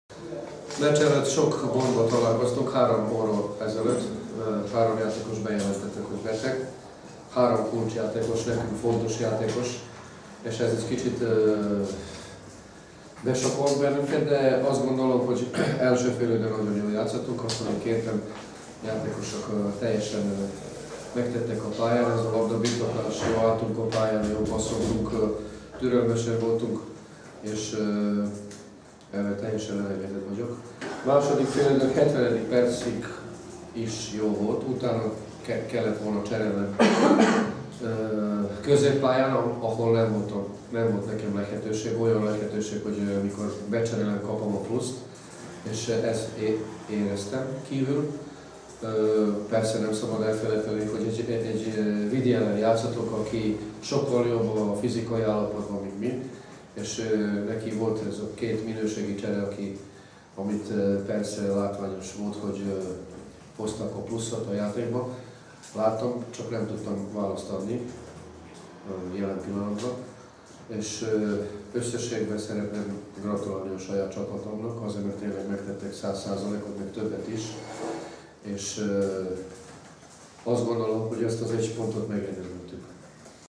MVM PAKS - Videoton FC sajtótájékoztató
Tomislav Sivic sajtótájékoztatója az MVM PAKS - Videoton FC (1:1) bajnoki mérkőzést követően. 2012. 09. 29.